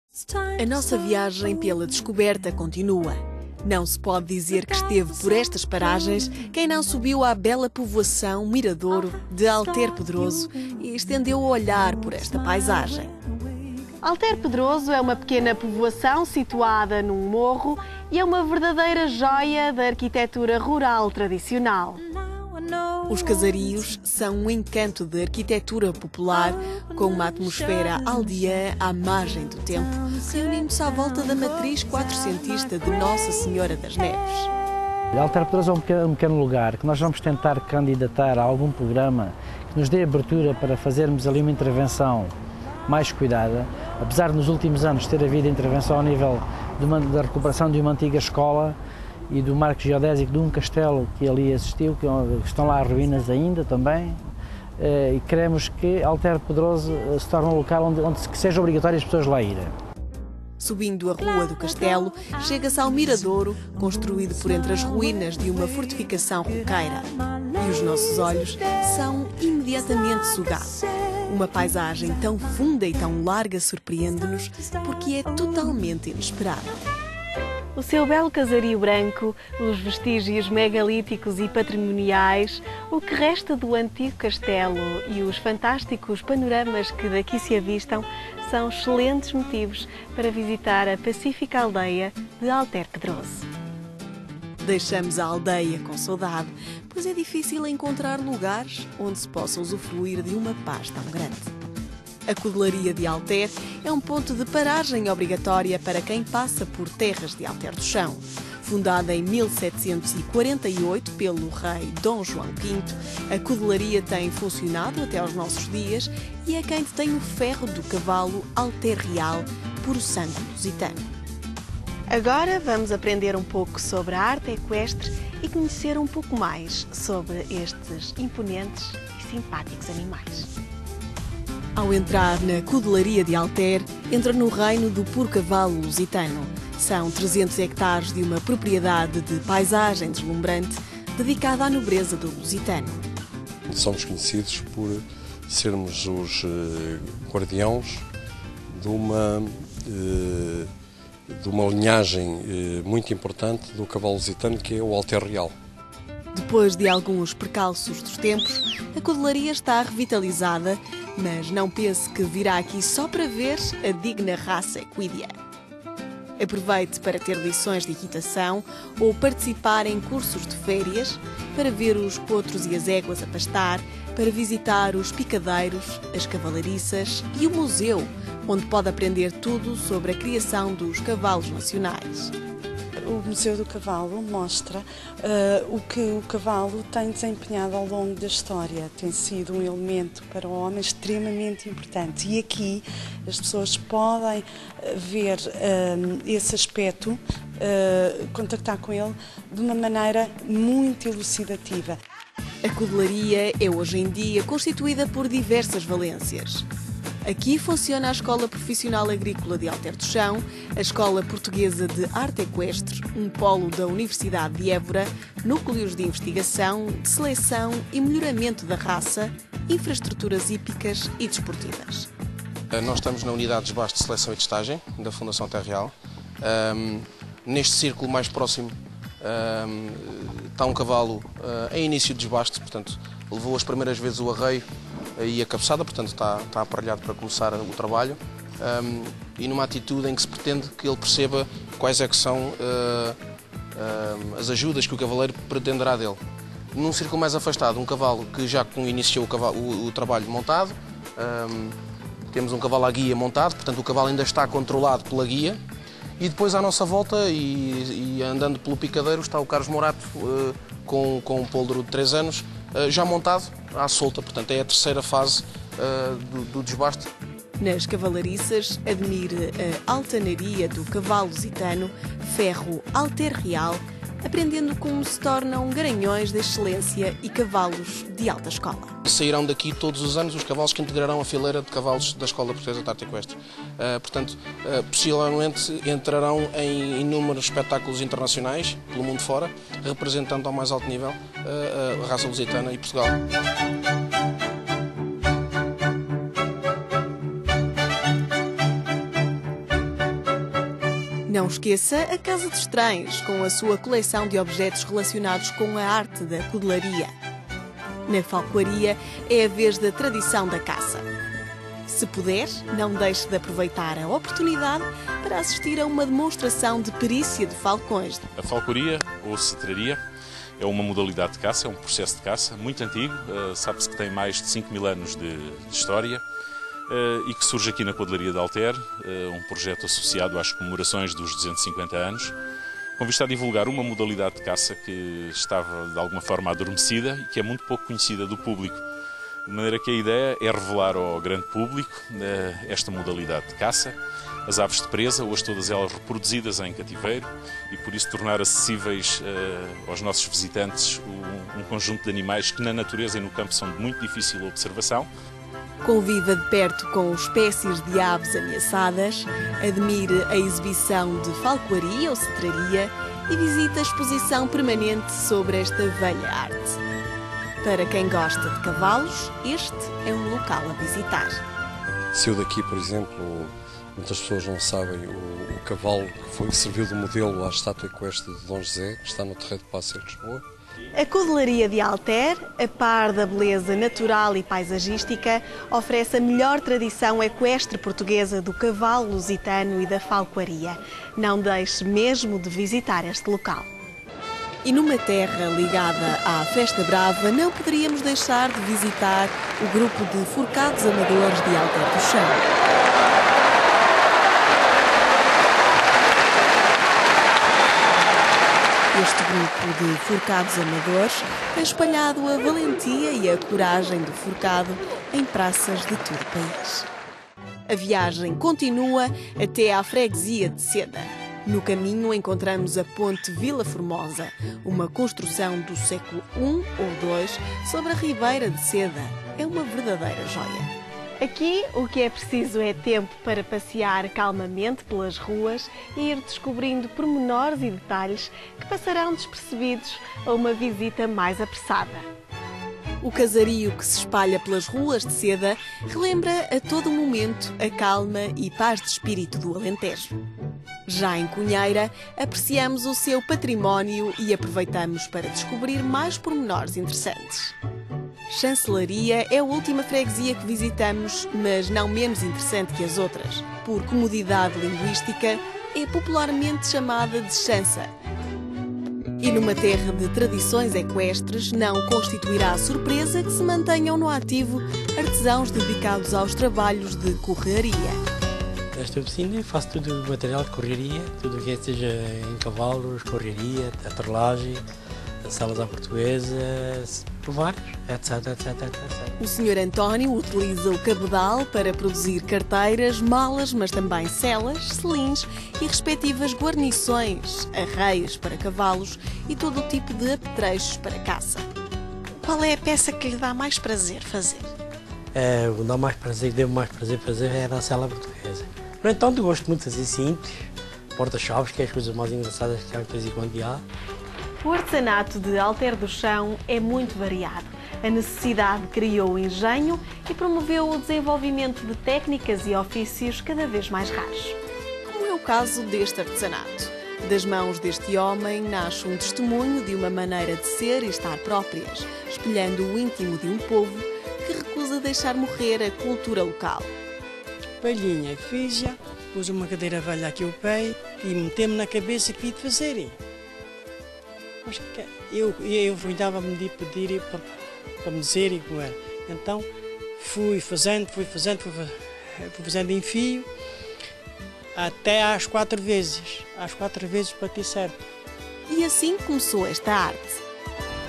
Alter do Chão Parte 2
O porque de visitar a bonita vila de Alter do Chão. Video em 3 partes a não perder.